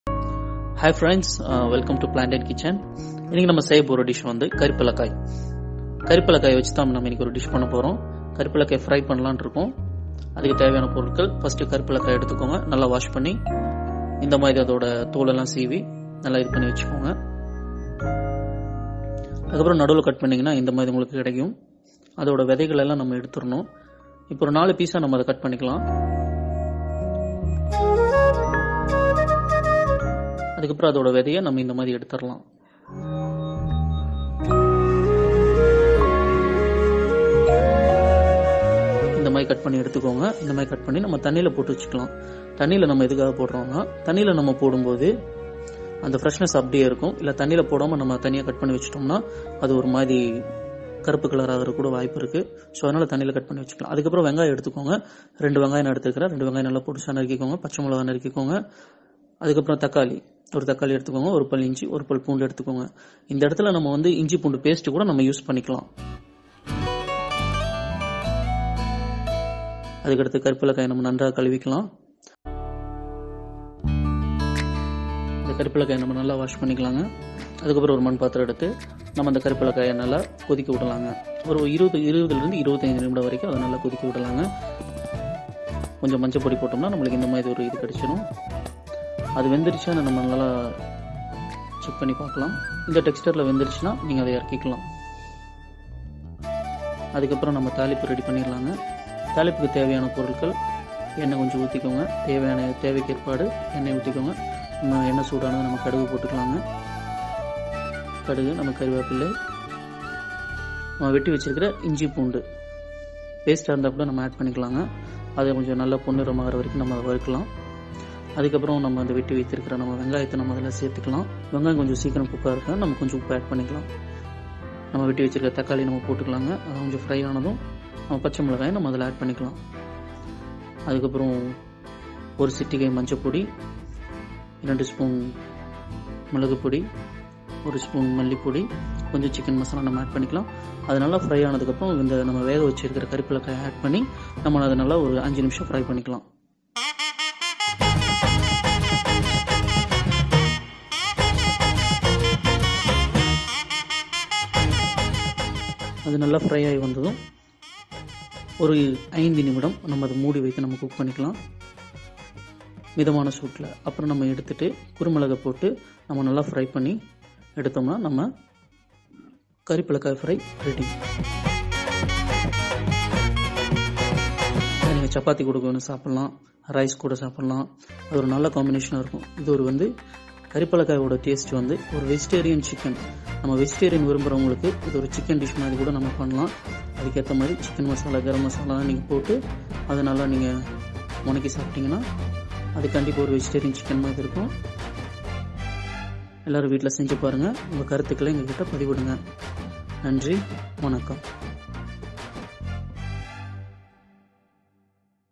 Karipalakai Pepper Fry
In this video we will show you how to make karpalakai pepper fry it is very easy an simple recipe just have a try